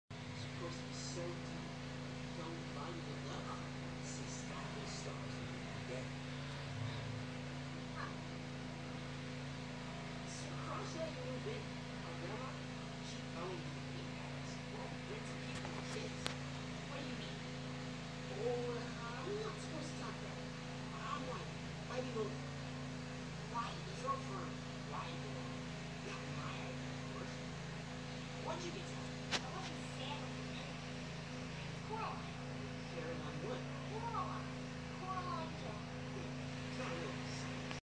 Netflix buffering unsatisfying
This is unsatisfying listening to a movie on Netflix without watching a movie it sucks and unsatisfying